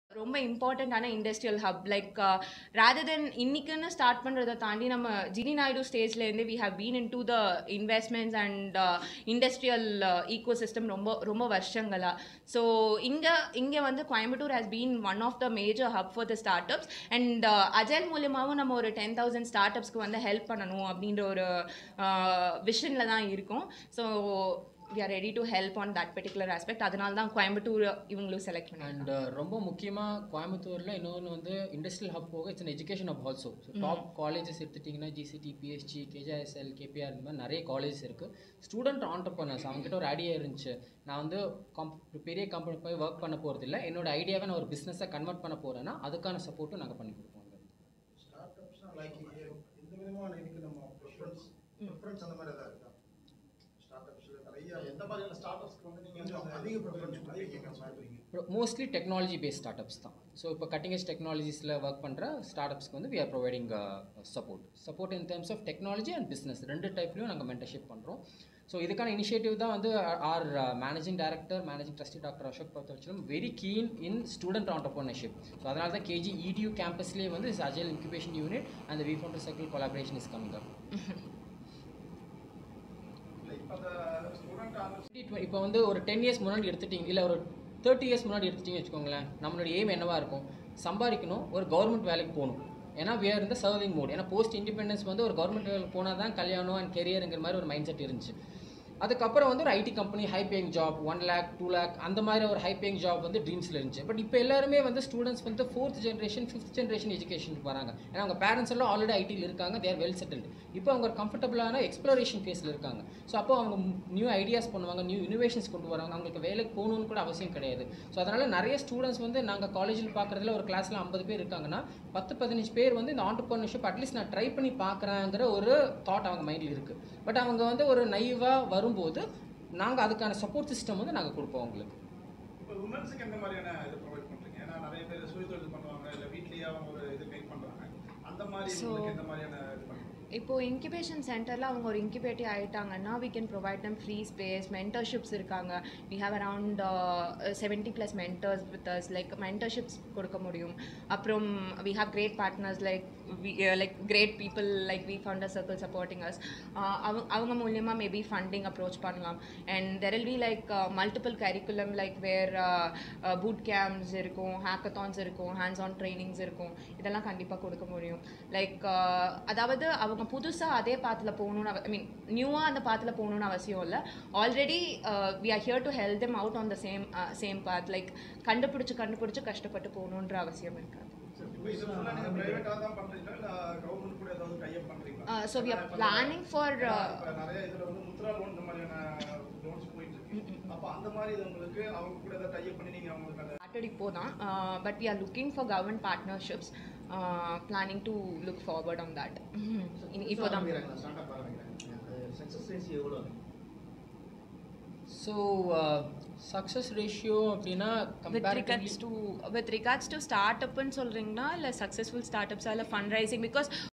கே.ஜி.ஐ.எஸ்.எல் கல்லூரியில் "அஜில்" இன்குபேஷன் மையம் துவக்கம்.
மாணவர்களை தொழில் முனைவோர்களாக மாற்றும் முயற்சியாக கோவை கே.ஜி.ஐ.எஸ்.எல் கல்லூரியில் "அஜில்" இன்குபேஷன் மையம் துவக்கம். இதன் வாயிலாக தொழில் துவங்கும் தொழில்முனைவோர்களுக்கு நிதியுதவி மற்றும் பயிற்சியளிக்க இருப்பதாக ஒருங்கிணைப்பாளர்கள் தெரிவித்துள்ளனர்….

கோவை சரவணம் பட்டி பகுதியில் உள்ள கே.ஜி.ஐ.எஸ்.எல் கல்லூரி மற்றும் விஃபவுண்டர் சர்க்கிள் இணைந்து - "அஜில்" ஸ்டார்ட்அப் இன்குபேஷன் சென்டரை தொடங்கியுள்ளது, இதற்கான துவக்க விழா கல்லூரி வளாகத்தில் நடைபெற்றது.இதில், வி ஃபவுண்டர் சர்க்கிள் இணை நிறுவனர்கள் தியோ சுரபா, பாவனா பட்நாகர், கே.ஜி.ஐ.எஸ்.எல் கல்வி நிருவனங்களின் தலைவர் டாக்டர் அசோக் பக்தவத்சலம், அஜில் இன்குபேஷ்ன் மையத்தின் தலைமை செயல் அதிகாரி சிவராகவி உள்ளிட்டோர் கலந்துகொண்டனர்.தொடர்ந்து செய்தியாளர்களிடம் இது குறித்து கூறுகையில், நகரத்தில் உள்ள ஸ்டார்ட்அப்களுக்கு உதவி மற்றும் பயிற்சி அளிக்கும் முயற்சியாக இதனை துவங்கியுள்ளதாகவும், சுமார் 10,000 ஸ்டார்ட்அப்களுக்கு உதவும் வகையில், ஒரு முறையான பாடத்திட்டம் மற்றும் திட்டத்தில் ஸ்டார்ட்அப் கட்டமைப்பைப் பற்றிய தொடர்புடைய தகவல்களை சேகரிப்பதை அஜில் நோக்கமாகக் கொண்டுள்ளதாக தெரிவித்தனர்... இதில் ஃபயர்சைட் கான்வர்சேஷன், பூட்கேம்ப்கள், ஹேக்கத்தான்கள், பிட்ச் புரோகிராம்கள், முதலீட்டாளர் ஸ்டார்ட்அப் மிக்சர்கள், பார்ட்னர் நன்மைகள் மற்றும் ரோட்ஷோக்கள் ஆகியவை அடங்கும்.
தொழில்முனைவோர்களின் பிரச்சனைகளை களைந்து அவர்களுக்கு ஒரு ஊக்கமும், பயிற்சியும் வழிமுறையும் வழங்கும் மையமாக செயல்பட உள்ள இதன் வாயிலாக , கல்லூரி மாணவர்கள் மற்றும் இளைஞர்களை வேலை தேடுபவர்களாக மாற்றாமல், வேலை வாய்ப்பை உருவாக்குபவர்களாக உருவாக்க வேண்டும். என்பதே நோக்கம் என தெரிவித்தனர்…